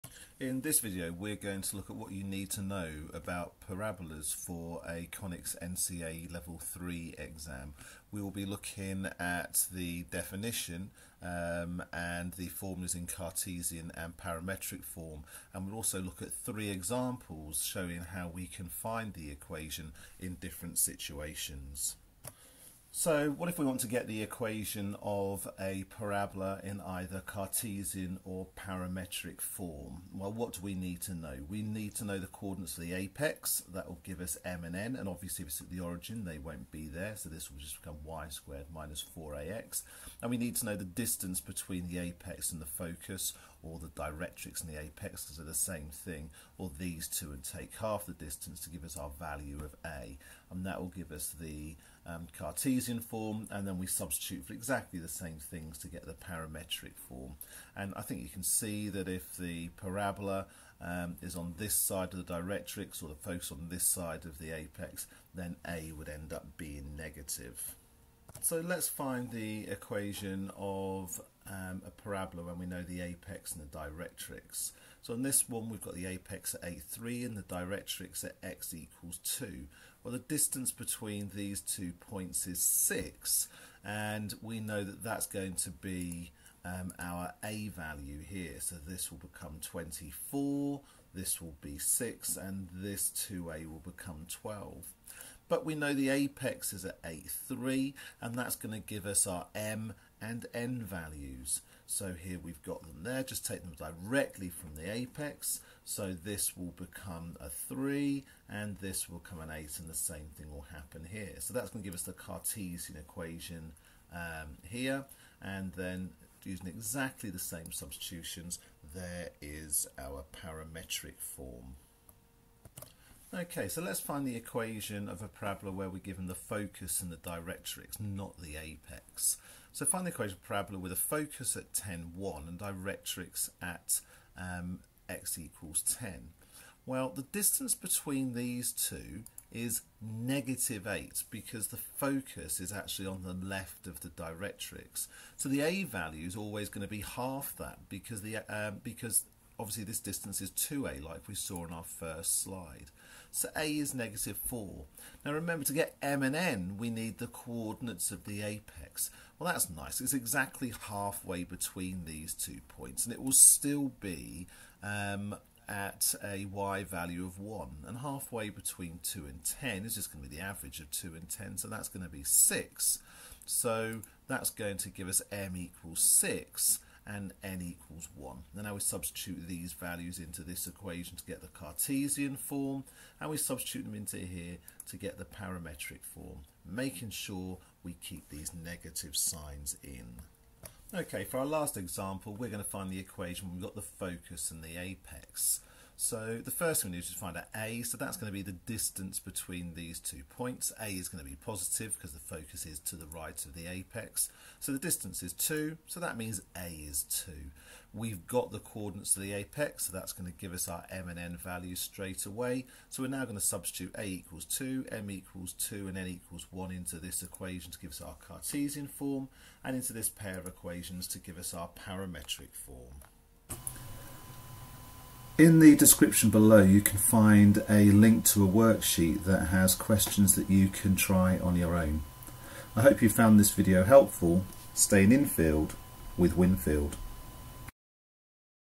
Conics Parabola
In this video, I show you what is required at NCEA level 3 for parabolas.  The examples here are all horizontally symmetrical will give an example in the worksheet of what to do when the parabola is symmetrical vertically,